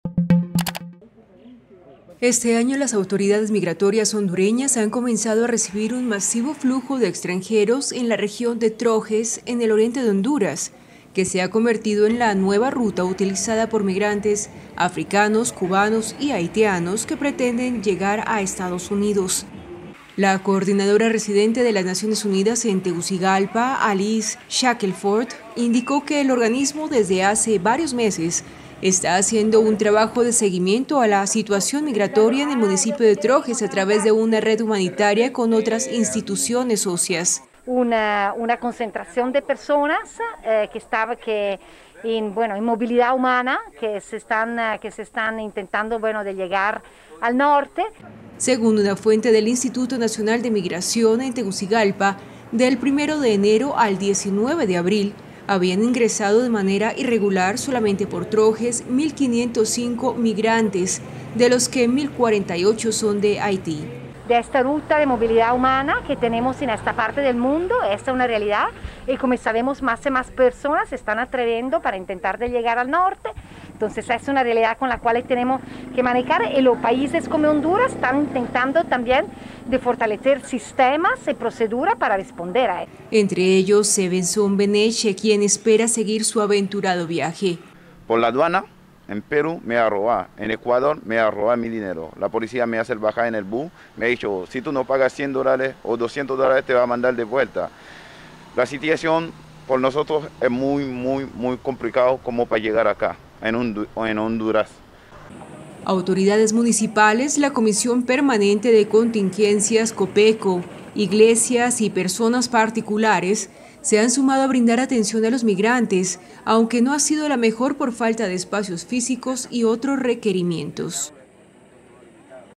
Migrantes abren una nueva ruta para ingresar a Honduras rumbo a Estados Unidos
La región de Trojes, en el oriente de Honduras, se ha convertido en una nueva ruta que migrantes africanos, cubanos y haitianos están utilizando para seguir hacia el norte con el objetivo de llegar a Estados Unidos, algunos después de hacer escalas en varios países sudamericanos.